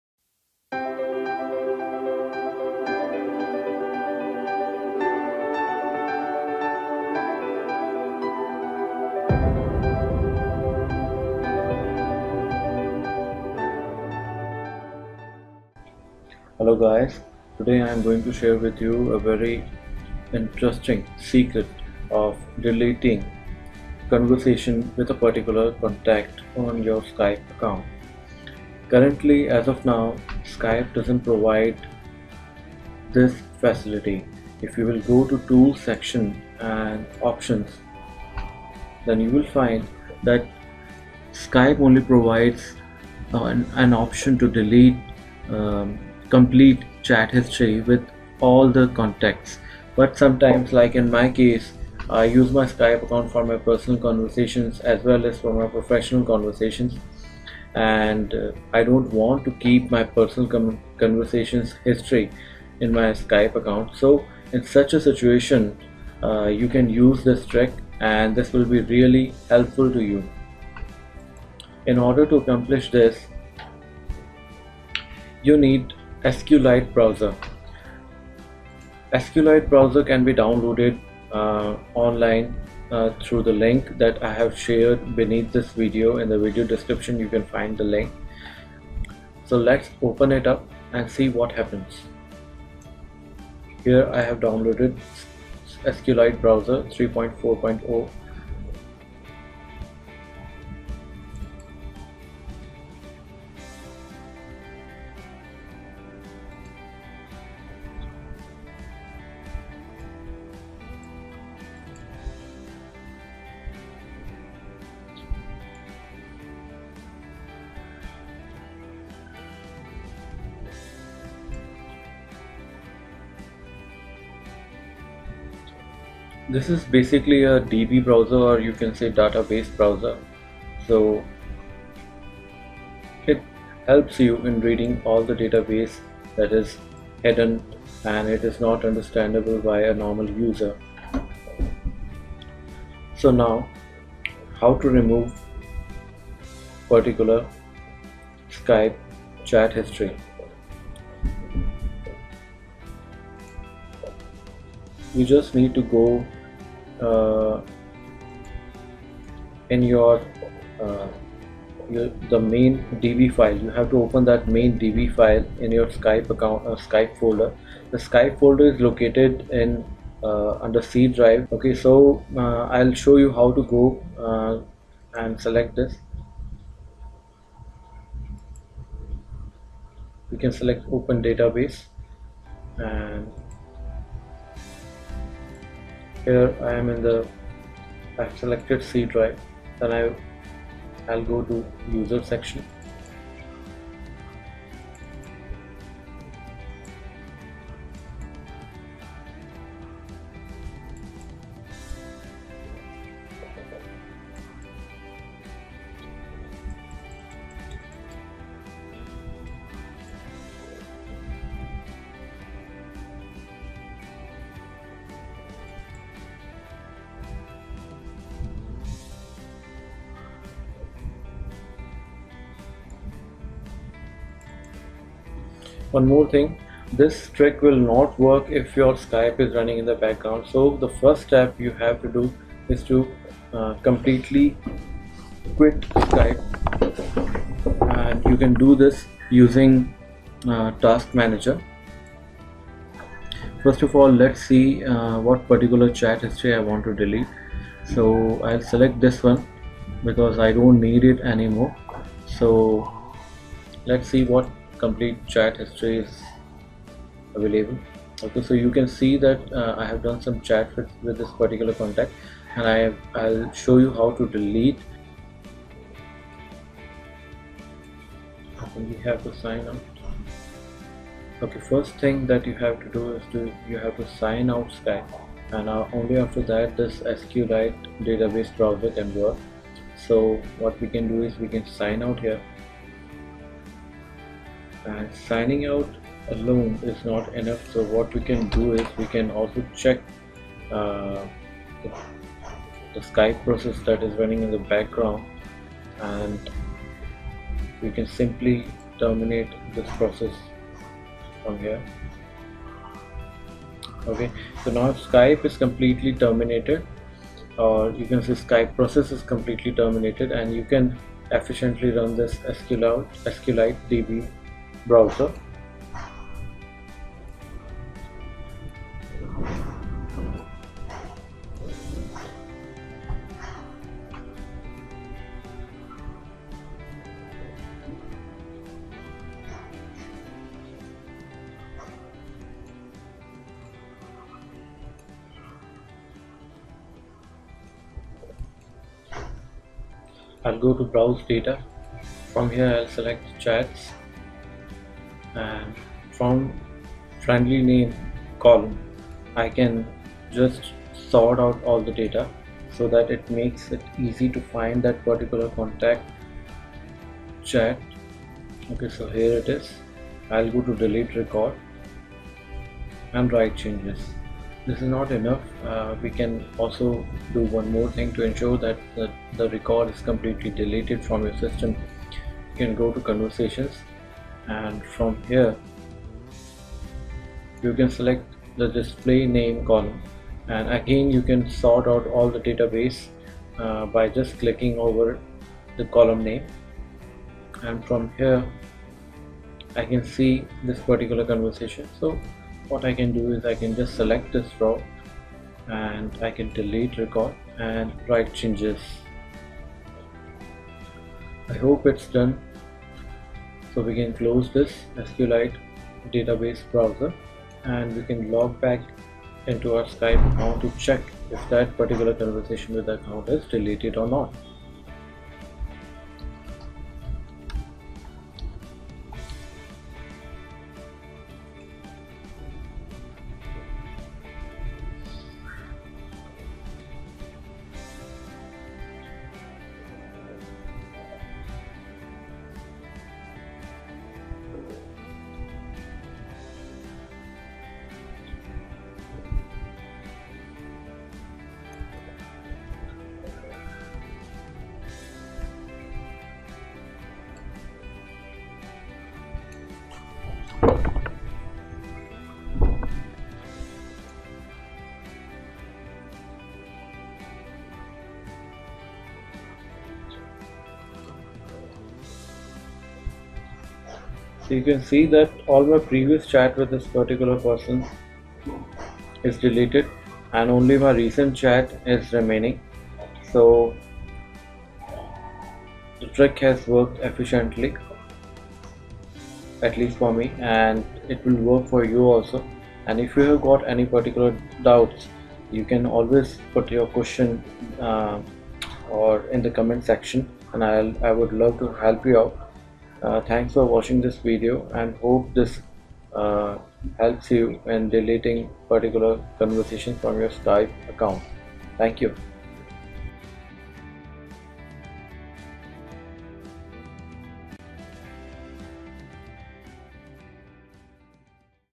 How to delete skype chat history for a particular contact
How to delete skype chat history for a particular contact only? I will share an easy trick with you in this video which will help you in deleting skype conversation with a specific contact only.